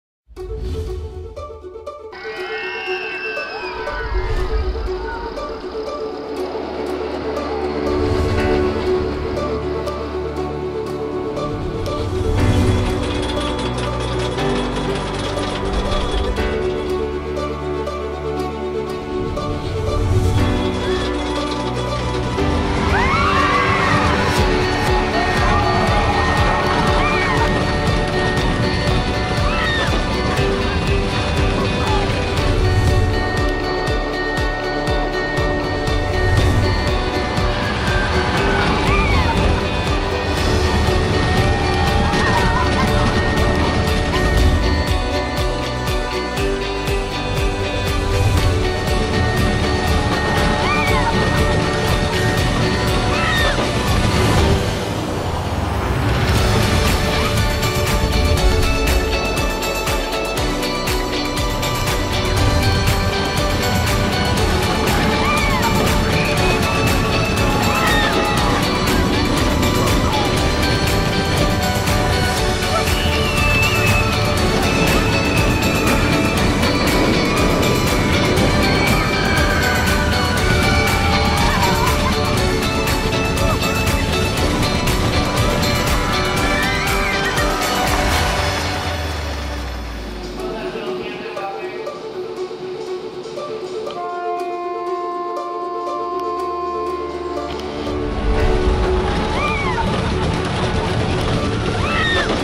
Twisted Colossus Construction Update #7
The lift hill has been replaced and much more!